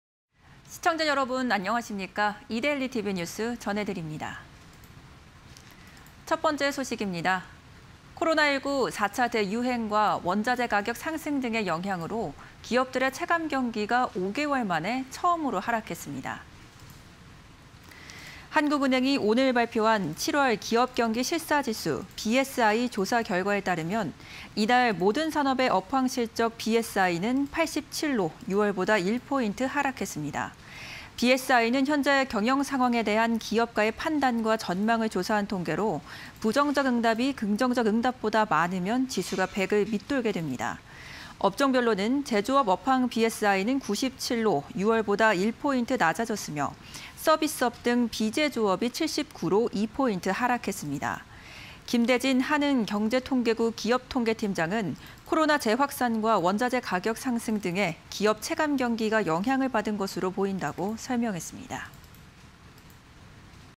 [이데일리N] 7월 기업 체감 경기 5개월만에 하락… 8월 전망지수도 6p↓(20210730)
코로나19 4차 대유행과 원자재 가격 상승 등의 영향으로 기업들의 체감 경기가 5개월 만에 처음으로 하락했습니다.
한국은행이 오늘 발표한 '7월 기업경기실사지수, BSI' 조사 결과에 따르면, 이달 모든 산업의 업황 실적 BSI는 87로 6월보다 1포인트 하락했습니다.
BSI는 현재 경영 상황에 대한 기업가의 판단과 전망을 조사한 통계로, 부정적 응답이 긍정적 응답보다 많으면 지수가 100을 밑돌게 됩니다.
업종별로는 제조업 업황 BSI는 97로 6월보다 1포인트 낮아졌으며, 서비스업 등 비제조업이 79로 2포인트 하락했습니다.
김대진 한은 경제통계국 기업통계팀장은 "코로나 재확산과 원자재 가격 상승 등에 기업 체감 경기가 영향을 받은 것으로 보인다"고 설명했습니다.